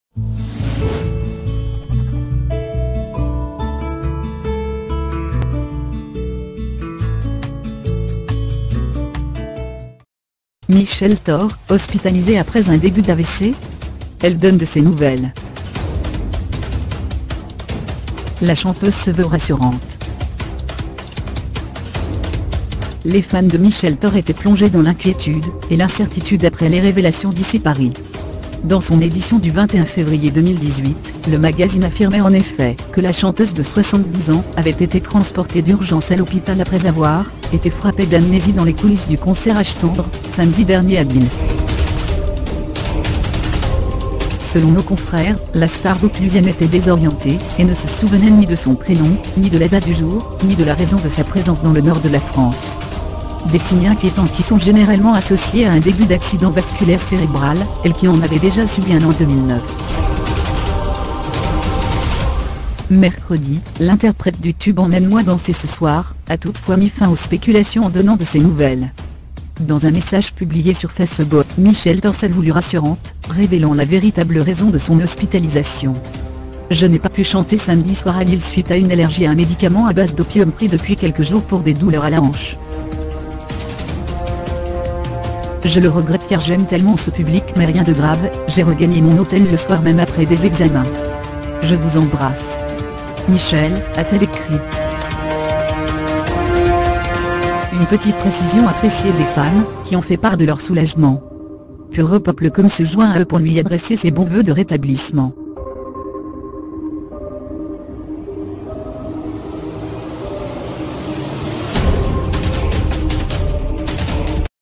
Michèle Torr, hospitalisée après un début d'AVC ? Elle donne de ses nouvelles
La chanteuse se veut rassurante.